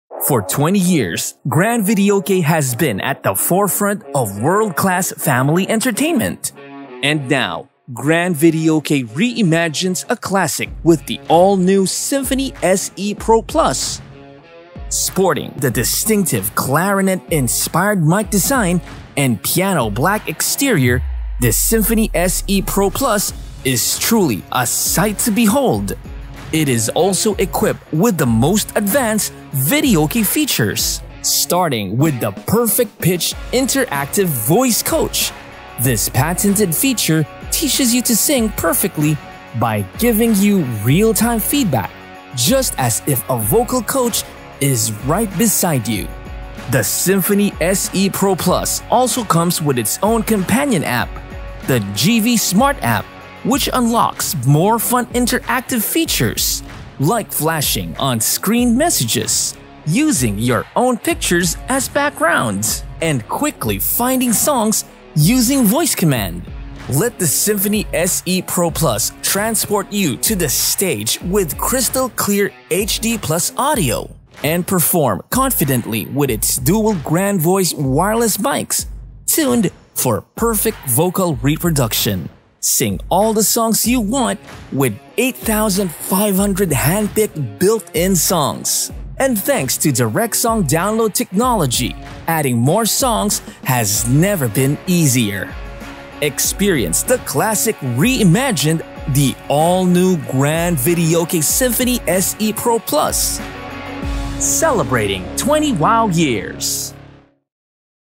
Grand Videoke Symphony SE Pro Plus: The Most Advanced Karaoke System
Celebrating 20 Years of Innovation for Families Worldwide.

The All-New SYMPHONY SE PRO PLUS represents the best of Grand Videoke’s rich heritage. From its iconic look to the MOST ADVANCED karaoke technologies, re-imagine what total family fun and entertainment should be with this latest masterpiece.